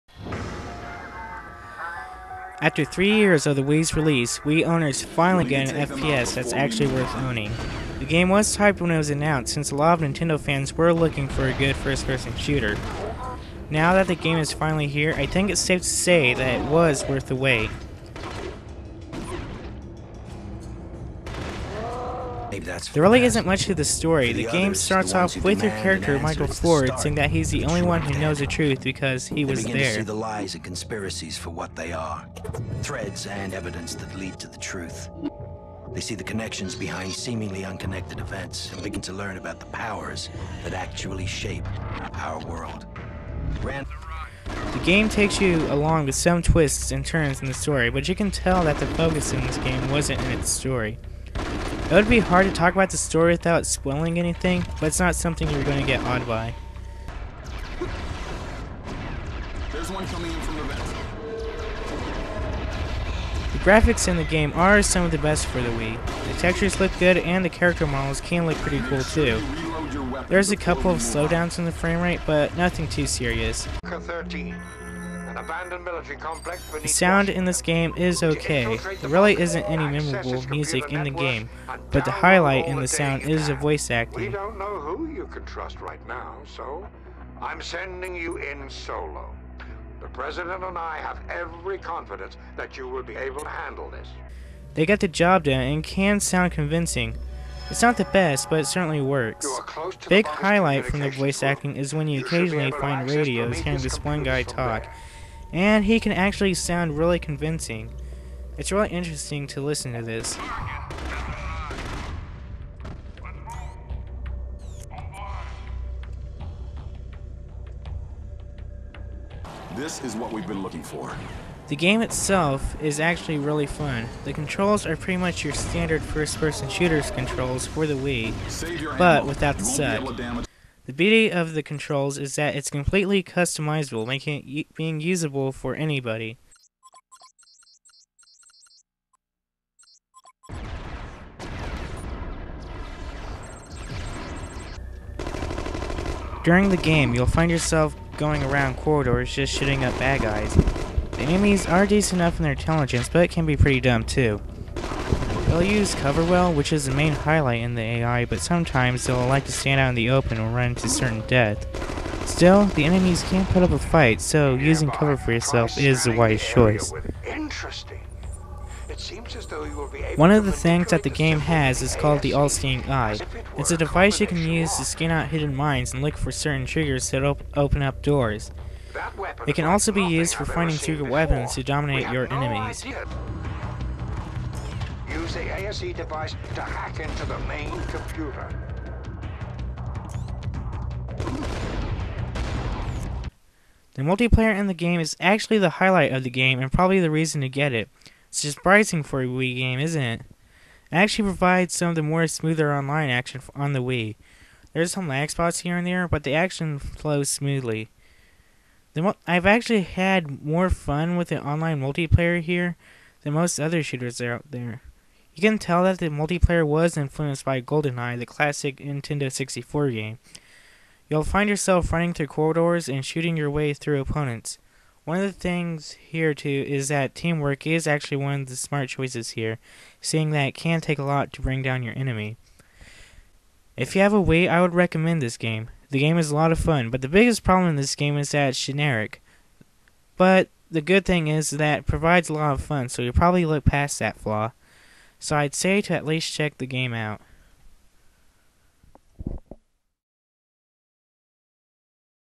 The Conduit Review
My first review EVAR. Well, I guess if it's one thing I need to be working on is my speaking. But other than that, I'm actually rather pleased on how this came out!